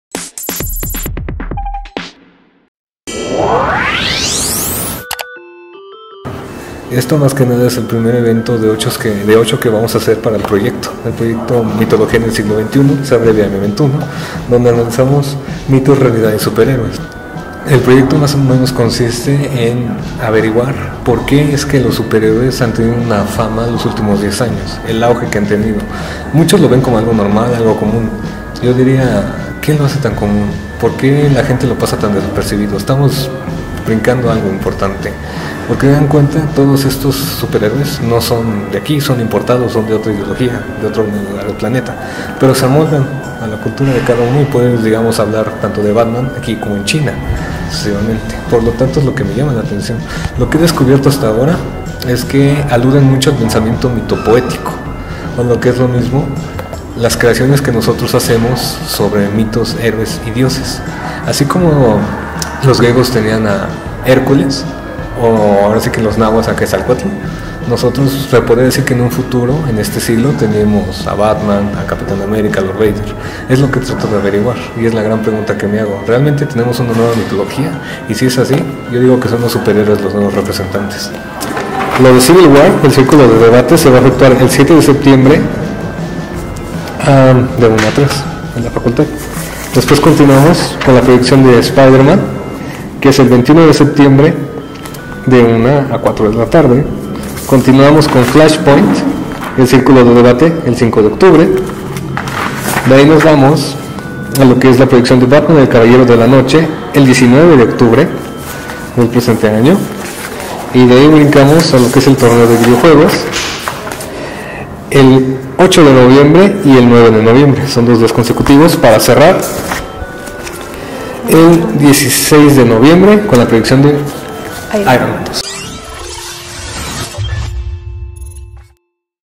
Los cómics: mitología del Siglo XXI
El cómic ha cobrado en los últimos quince años un auge sin precedentes debido a que en el siglo XXI los héroes de Marvel y DC formaron parte de una nueva mitología. “Así como los griegos tenían a Hércules o a los Mexicas o a Quetzalcóatl, en este siglo tenemos a Batman y Superman”, plantea Erick Alejandro Gutiérrez Flores, pasante de sociología de la Facultad de Ciencias Políticas y Sociales (FCPyS) de la UNAM.